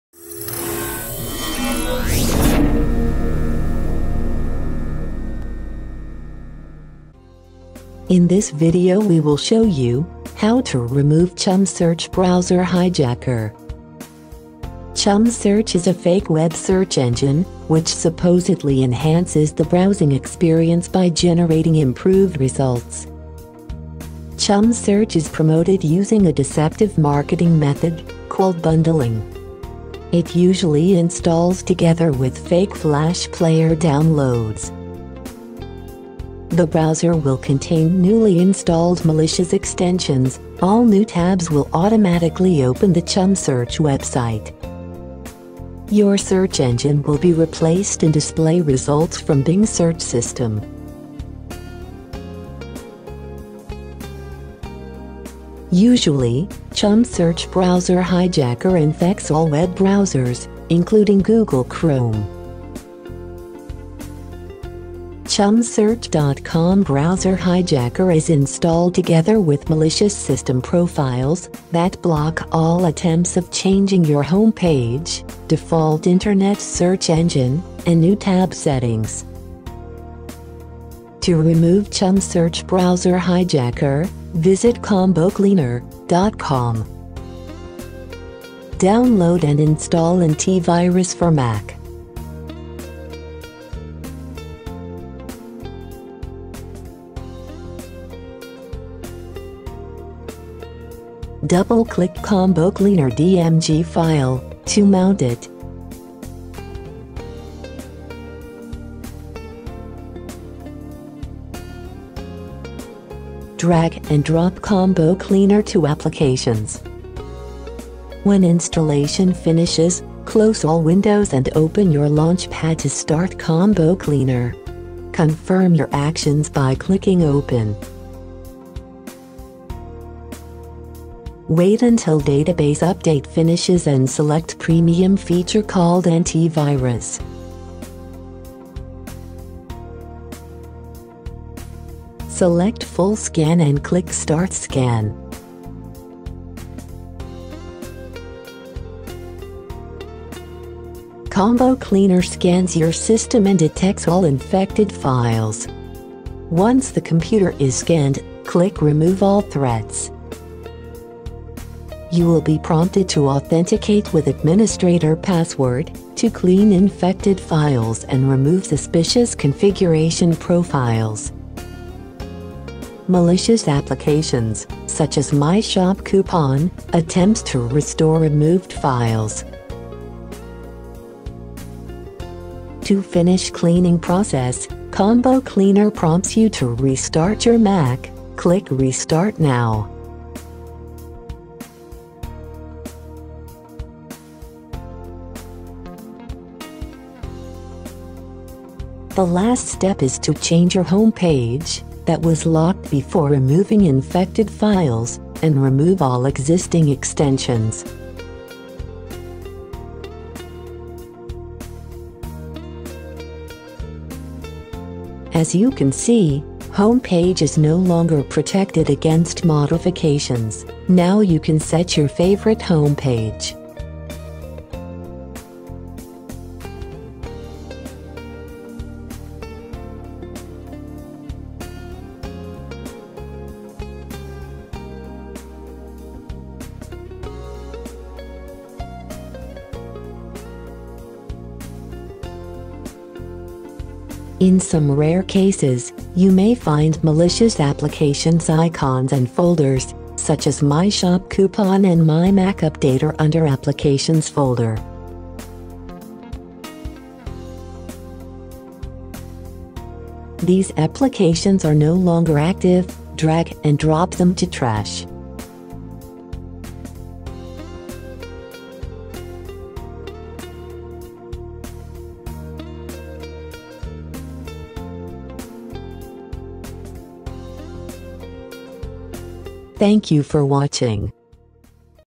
How to remove Chumsearch.com browser hijacker using Combo Cleaner
Video showing how to remove Chumsearch.com browser hijacker using Combo Cleaner